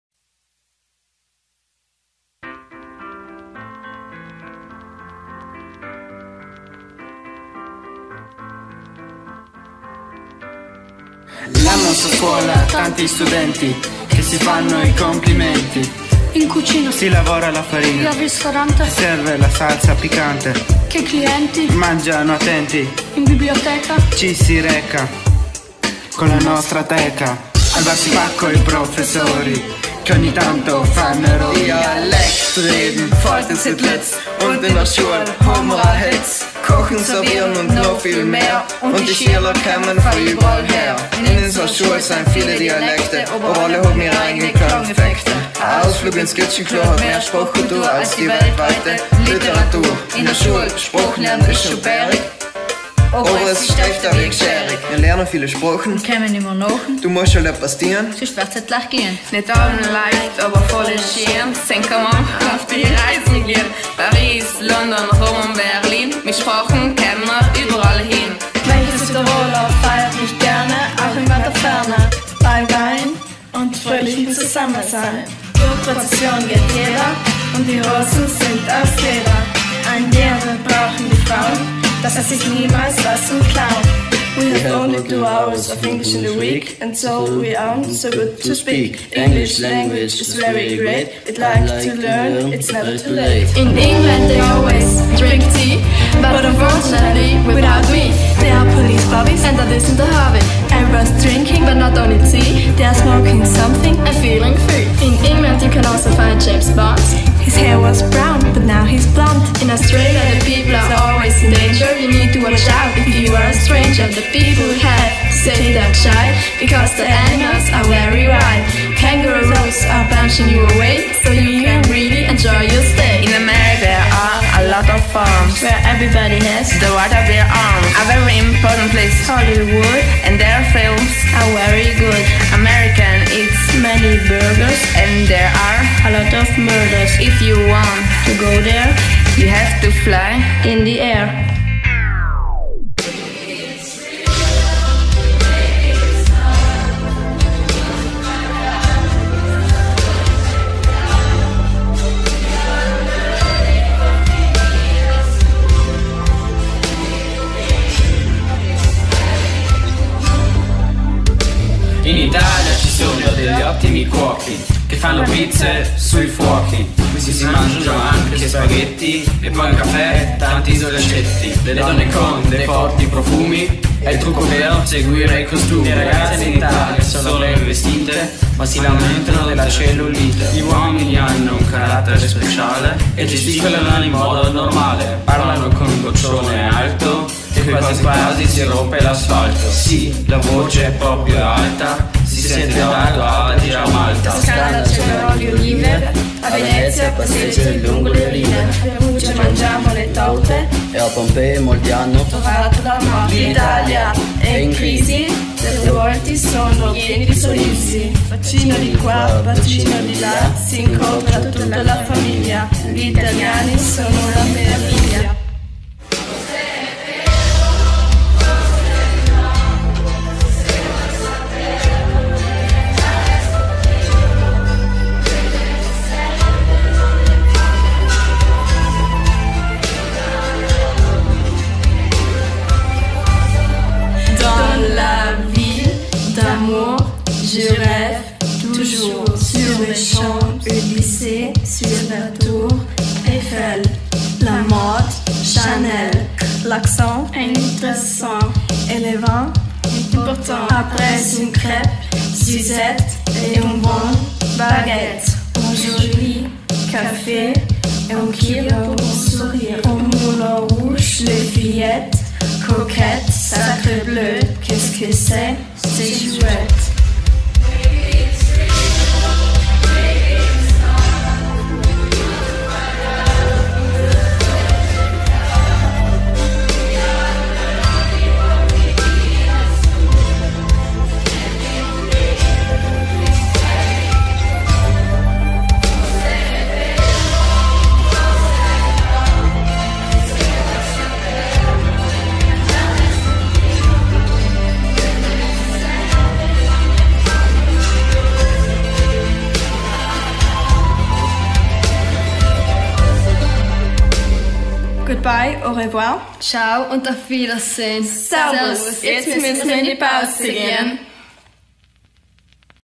FIND OUT - Landesberufsschule "Emma Hellenstainer"
Unser Beitrag zur Aktion "Europäischer Tag der Sprachen" der Autonomen Provinz Bozen.
Mitgewirkt haben die Klassen 1 A Hofa, 1 B Hofa, 1 B FS Kochen und 1 FS Service im Schuljahr 2015/16 unter der Gesamtleitung von Emanuele Colombi (Englischlehrer).
Ein Dank für die Unterstützung an die Lehrpersonen Patrizia Solaro, Fabrizia Ruggiu, Waltraud Kompatscher und Petra Plankl.